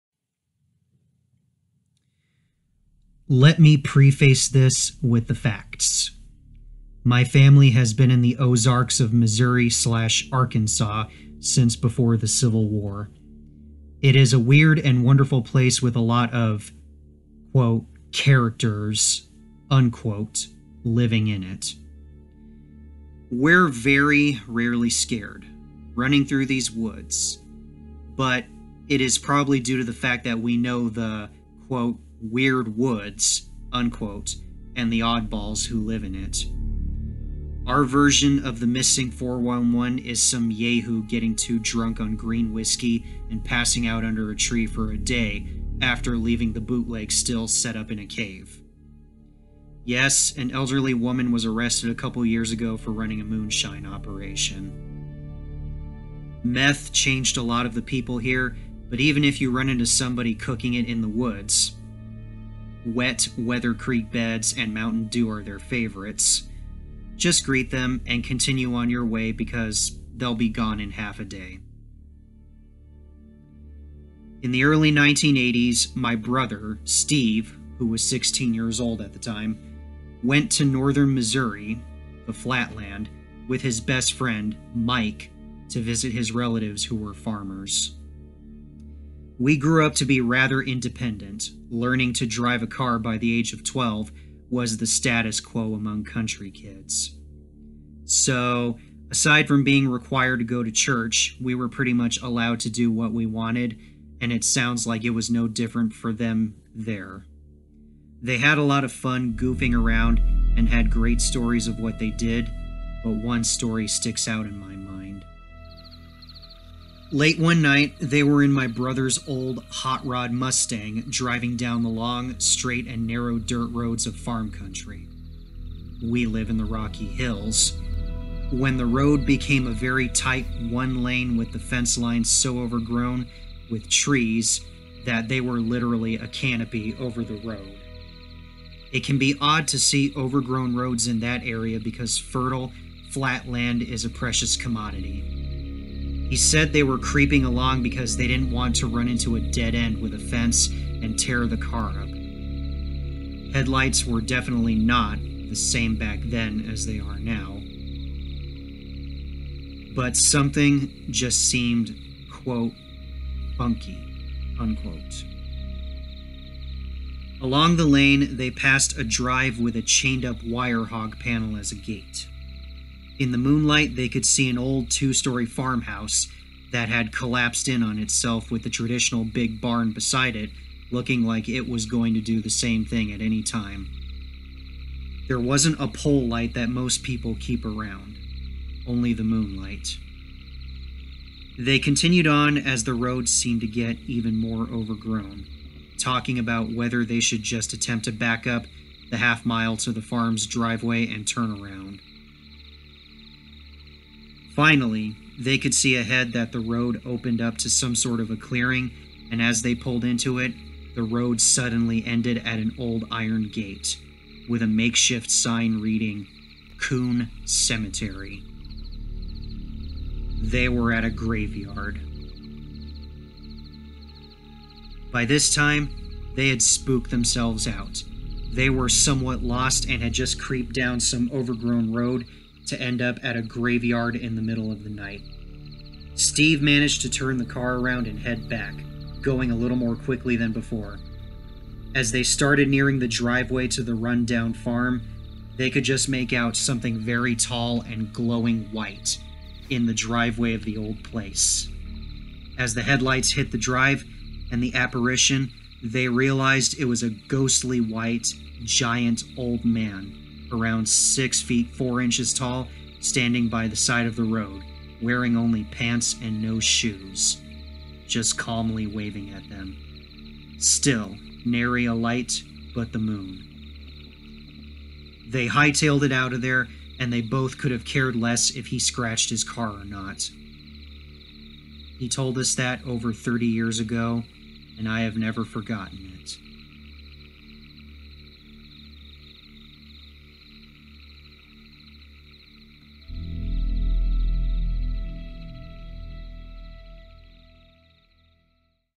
[Scary Story Reading] Leaving The Woods You Know - A horror story from the Ozark area of Missouri
Man, this story was some really creepy-ass shit. I mean seriously, I know it took place in the 1980s, but how the hell can you get lost on roads that you should've been familiar with, only to end up in an area with a worn down and abandoned farmstead, and a spooky as hell cemetery in it's vicinity?

Furthermore, what genius thought to put a cemetery in the middle of fucking nowhere?

That's some wierd shit bro.